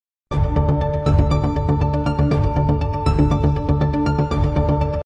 Qarabağın futbolçusu Rahil Məmmədov rəsmən Polşa Ekstraklasa klubunda / Video - 2024 /
Kanalda  yerləşən xəbərlərə və şərhlərə görə yalnız müəlliflər məsuliyyət daşıyır.